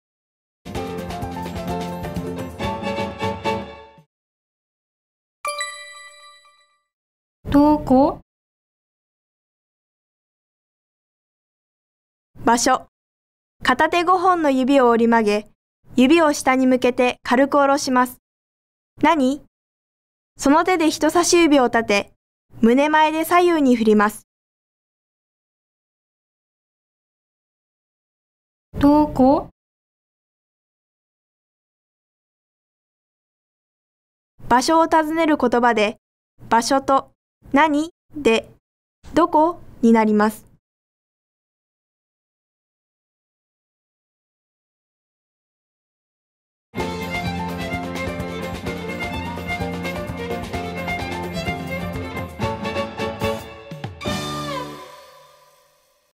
手話「どこ？」
手話を動画で紹介します。手話を覚えて、表してみましょう。
大府市手話紹介動画